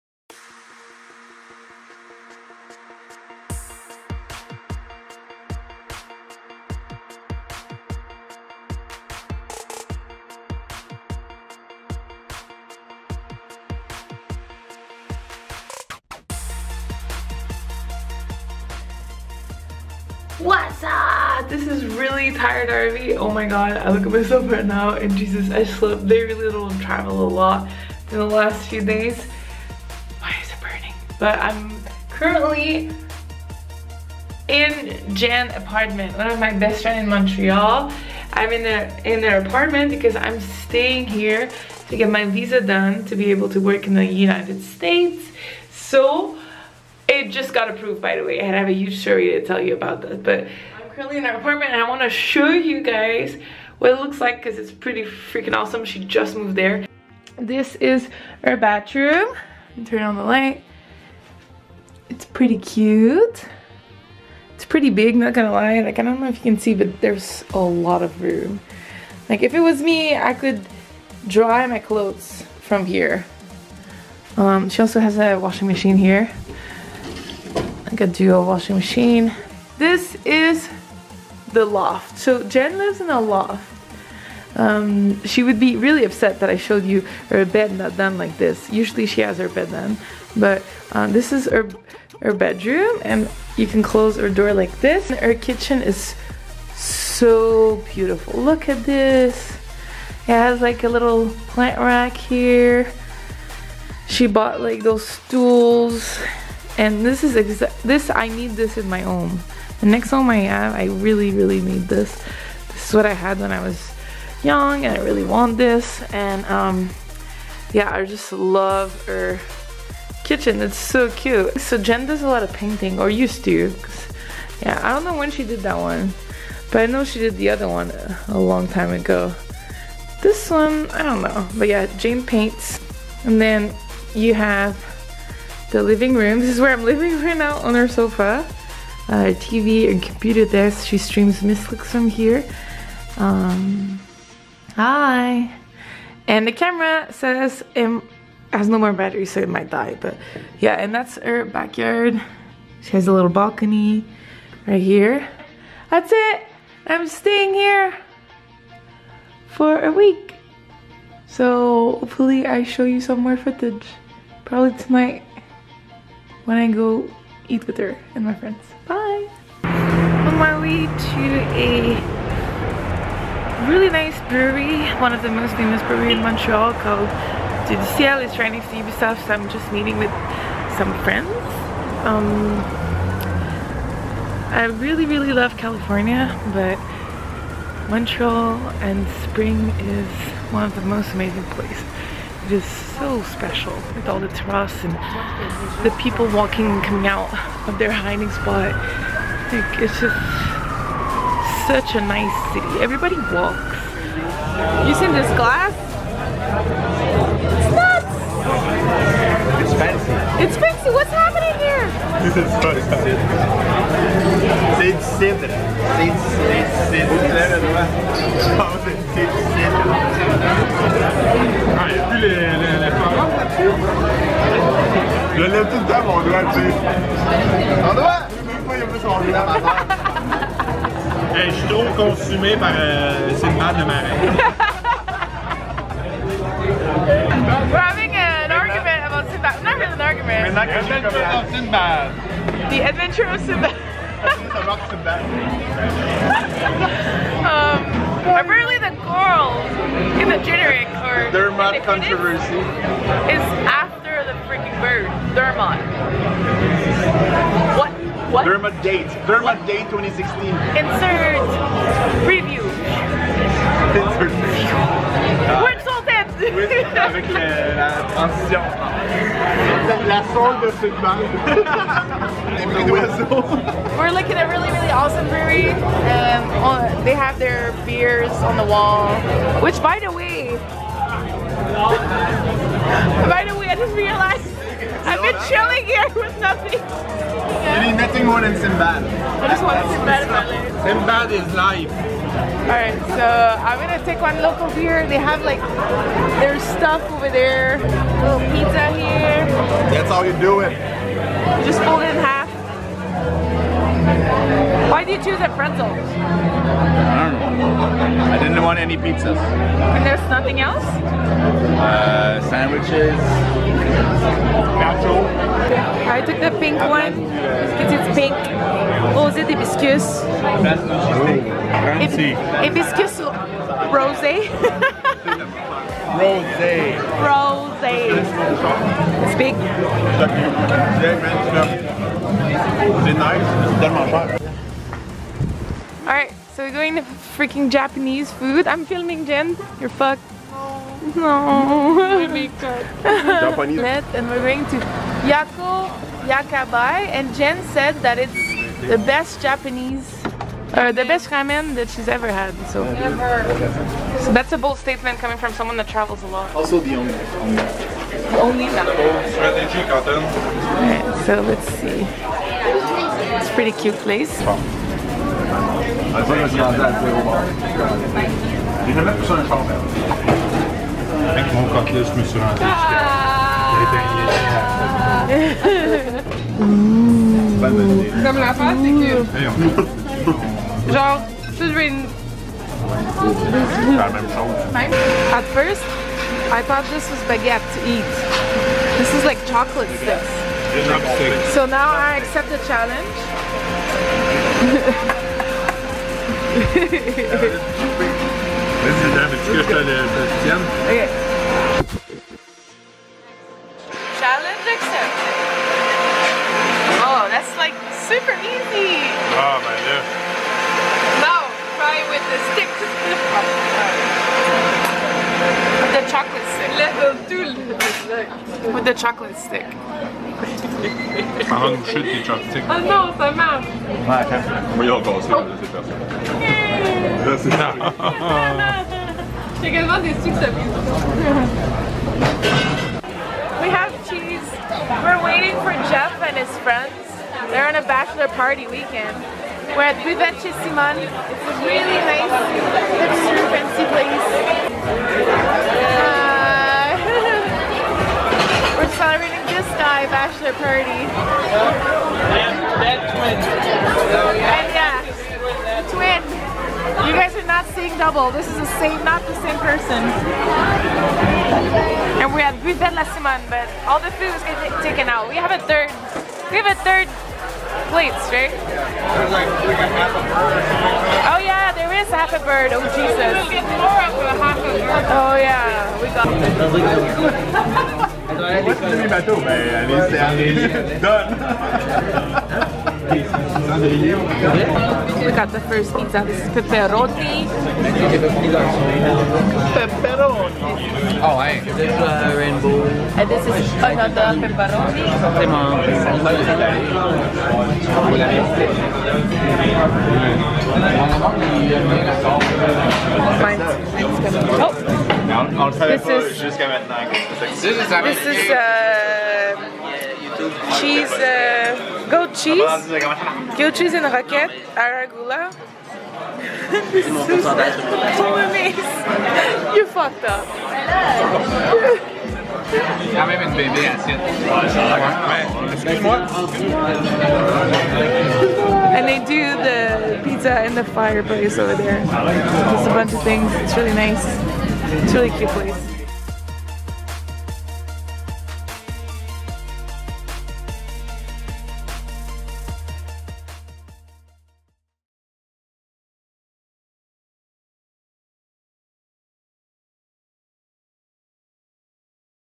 Restaurant Hopping in Montreal
Restaurants featured:

Dieu du Ciel
Yokato Yokabai
La Buvette chez Simone
Magpie